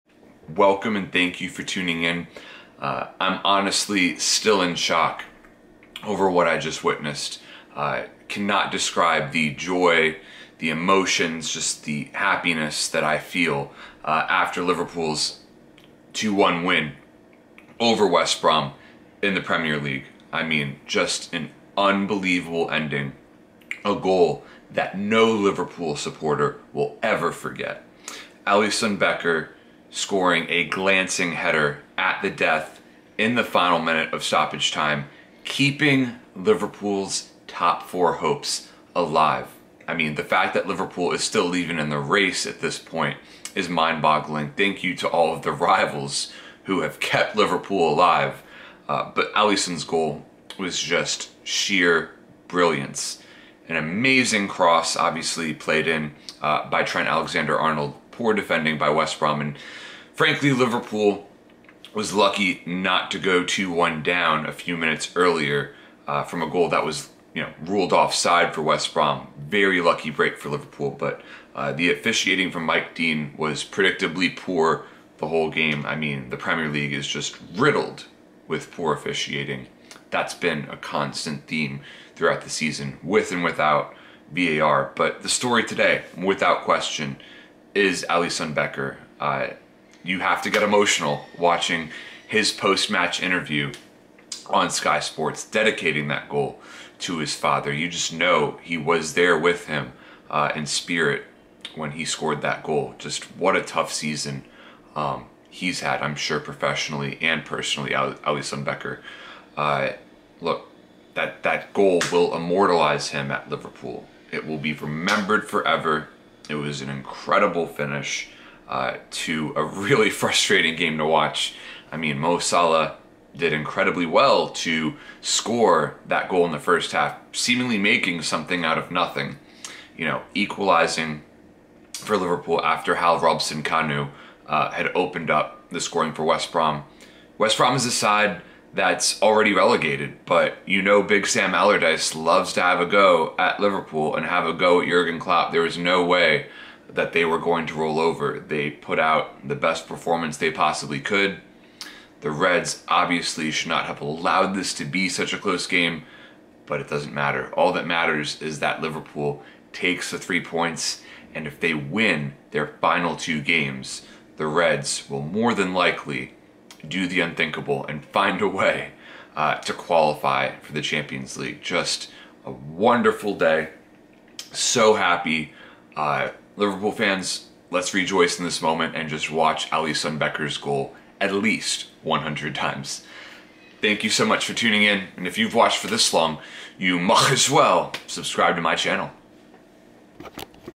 ALISSON KEEPS TOP 4 HOPES ALIVE WITH UNBELIEVABLE GOAL!
Alisson Becker scored the first goal by a goalkeeper in LFC history with a glancing header at the death, giving Liverpool a 2-1 win over West Brom.